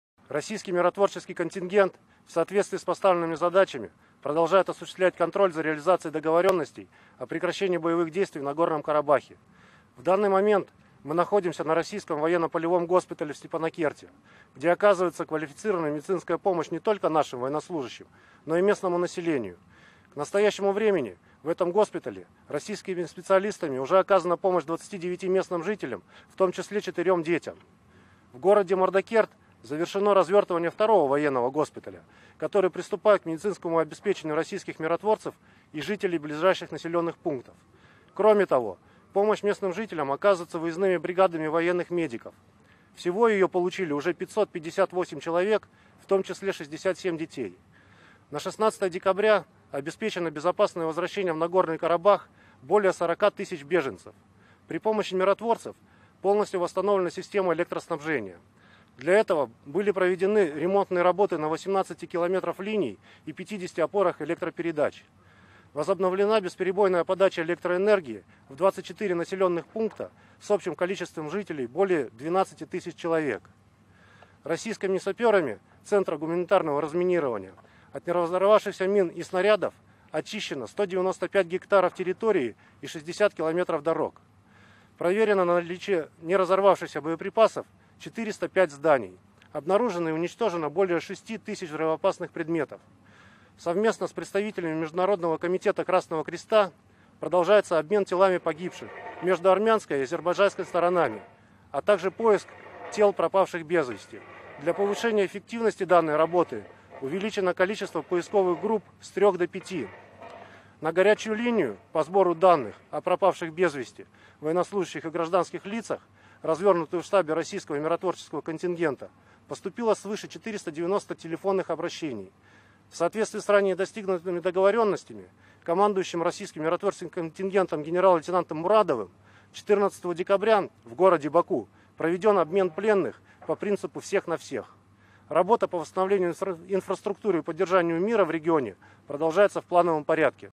Брифинг по обстановке в Нагорном Карабахе
Российский миротворческий контингент в соответствии с поставленными задачами продолжает осуществлять контроль за реализацией договоренностей о прекращении боевых действий в Нагорном Карабахе.